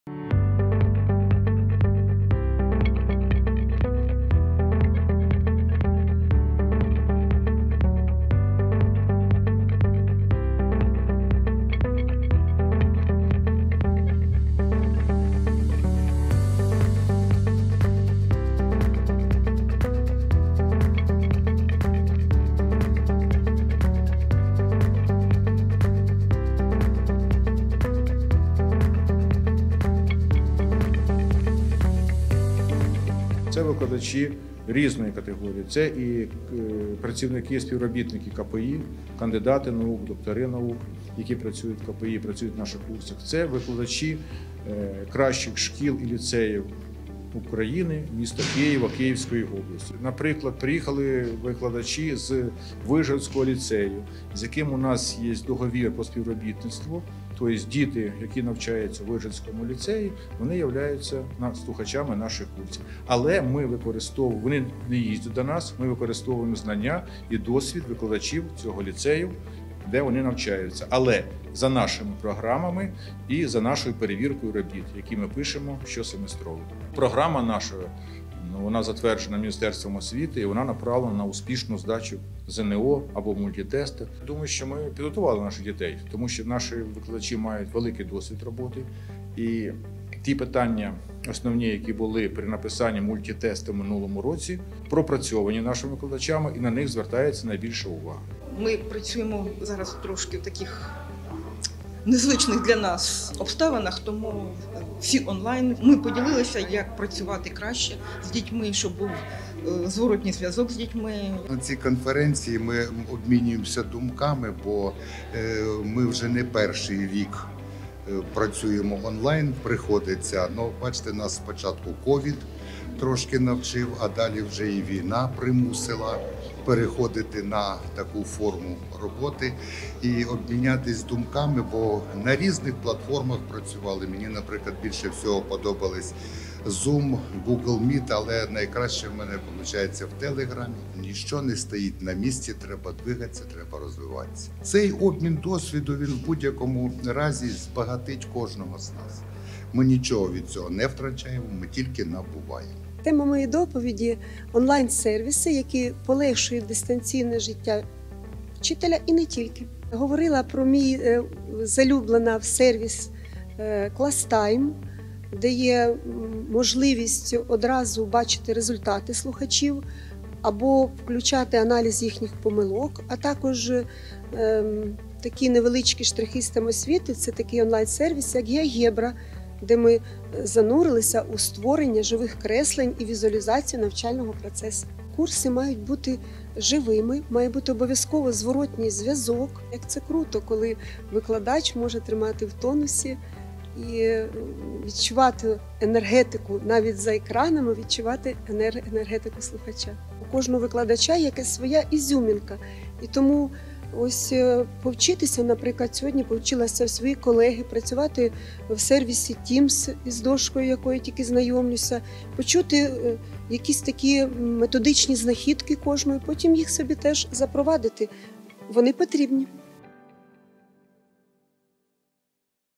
Вступ в умовах війни
У КПІ готують абітурієнтів до вступу в українські виші, передовсім — до КПІ. Серед слухачів курсів — вступники з України, Великої Британії, Франції, Німеччини, Польщі. Програма підготовчих курсів затверджена МОН і спрямована на успішне складання ЗНО і НМТ.

Київська політехніка зібрала викладачів підготовчих курсів з Києва та області на конференцію для обміну думками й досвідом.

📋 Докладно про підготовчі курси КПІ — за посиланням.